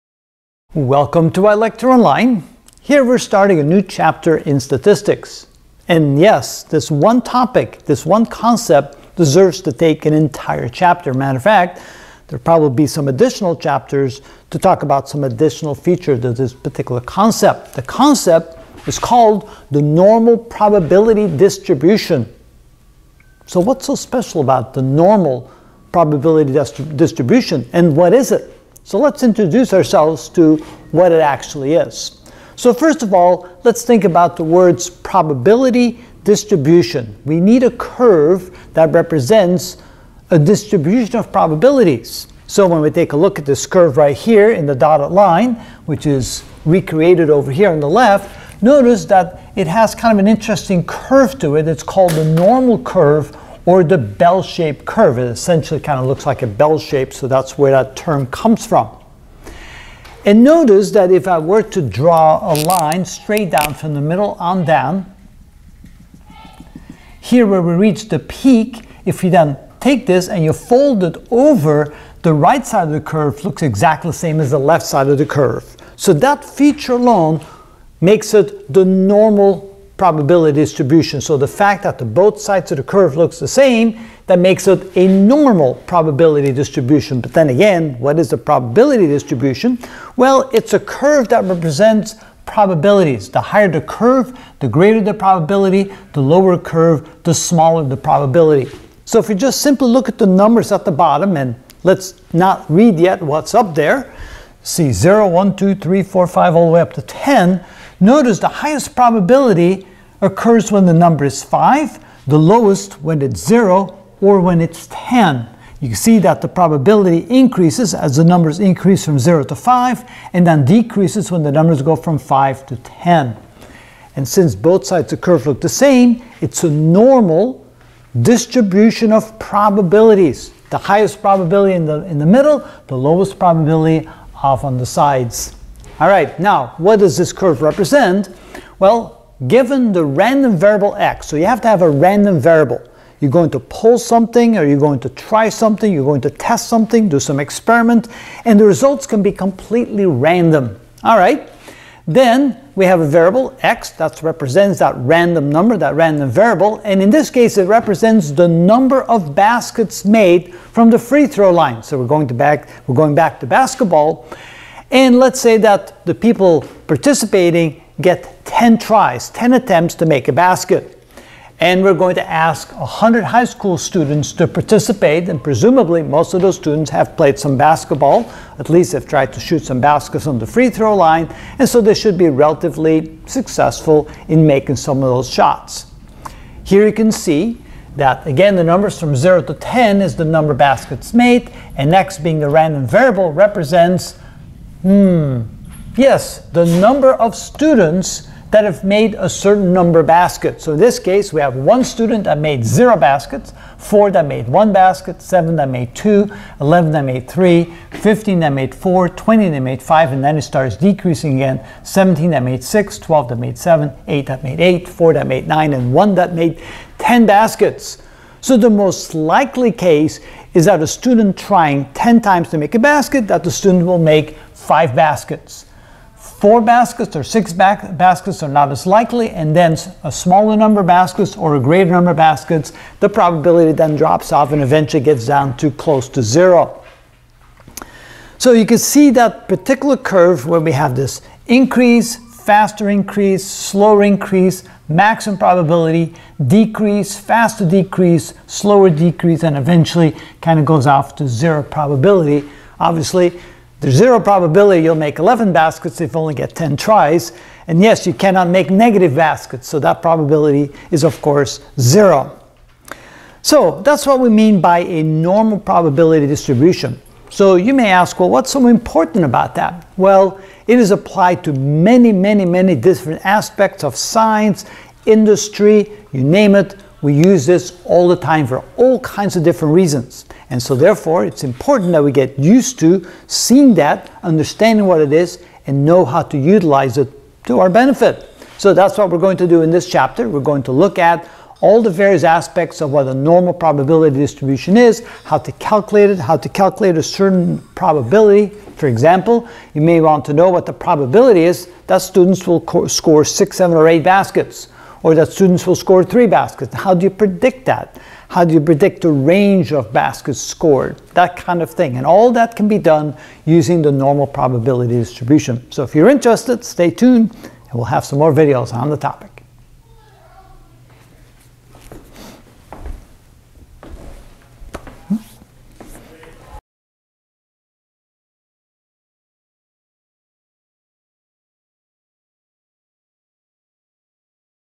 Statistics: Ch 6 The Normal Probability Distribution (1 of 28) What is the Normal Probability Dist.?
We will learn what is a normal probability distribution using an example. Given: the random variable x, where x represents the number of baskets made from the free throw line out of 10 attempts (n=10). We are asking 100 high school students to participate.